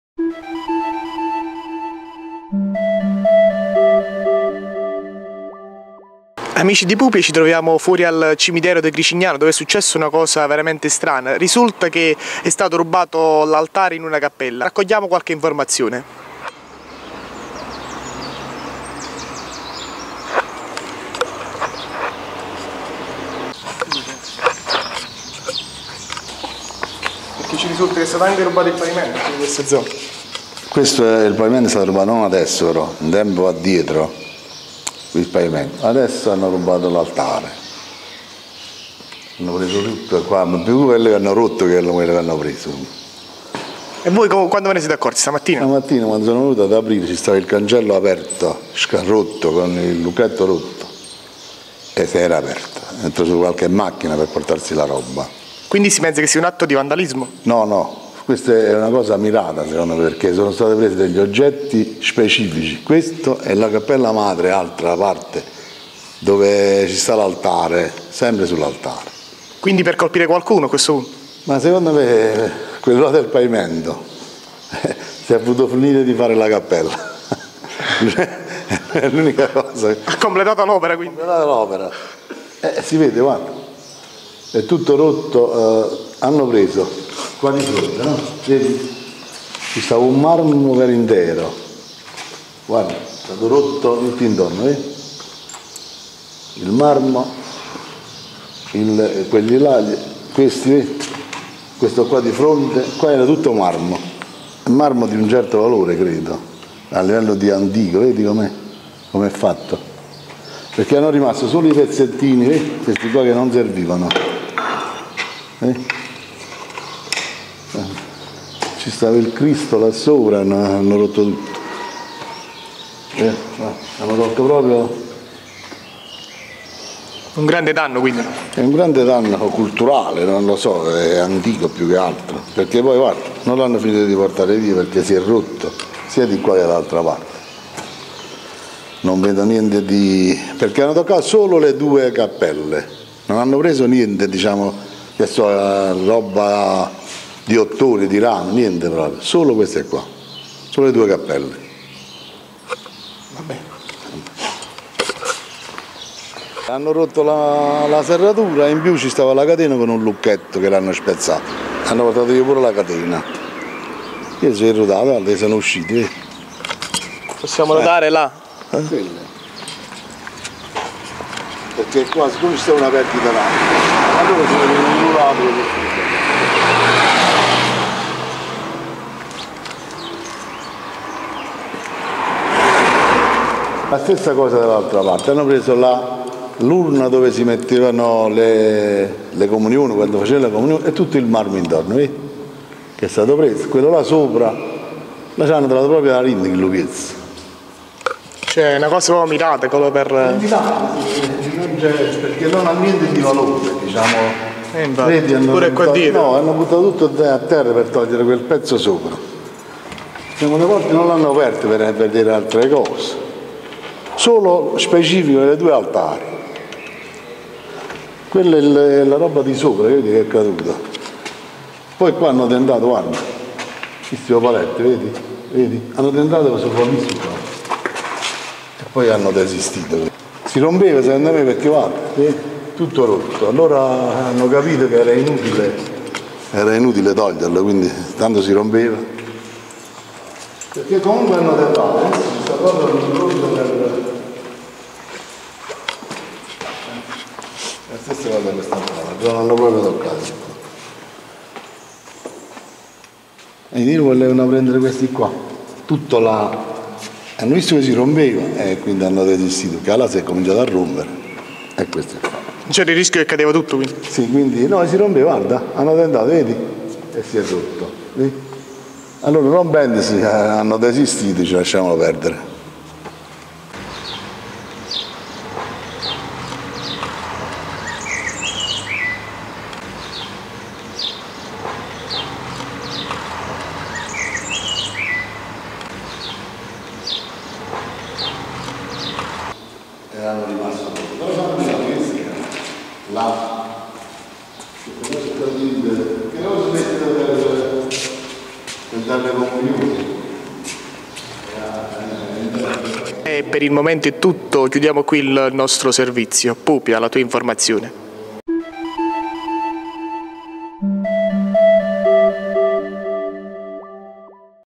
Gricignano (CE) - Furto sacrilego al cimitero: "spogliati" gli altari delle cappelle (06.06.12)
- Gricignano (CE) - Furto sacrilego al cimitero: "spogliati" gli altari delle cappelle. Due altari interamente "spogliati" dell'antico marmo. E' successo, la scorsa notte, nel cimitero di Gricignano, dove ignoti hanno compiuto il furto sacrilego nella cappella madre e in quella del Purgatorio. Entrando all'interno del luogo sacro dall'accesso secondario sulla provinciale Gricignano-Succivo, a bordo di una vettura, come testimoniano il lucchetto del cancello forzato e le tracce di pneumatici rinvenute sul posto, i malviventi si sono introdotti nelle due cappelle, dove già in passato si era verificato il furto dei pavimenti, e hanno asportato il marmo dagli altari, per poi allontanarsi.
L'episodio è stato scoperto la mattina successiva dal custode del cimitero, Giuseppe Moretti, che ha allertato le autorità. Sul posto, per i rilievi, i carabinieri della locale stazione e il responsabile dell'ufficio tecnico, ingegner Ernesto Palermiti. Resta l'interrogativo sul fatto che si tratti di un mero furto oppure di un'azione mirata. (06.06.12)